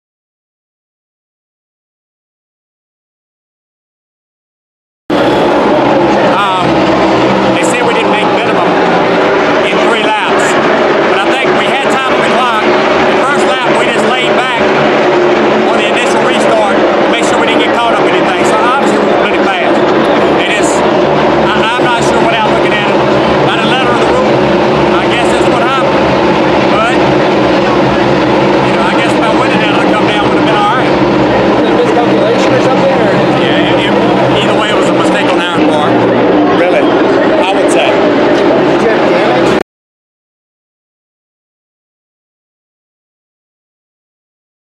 Bootie Barker Reaction Interview
Crew Chief Bootie Barker explains what ended @BubbaWallace
night early at the Coke600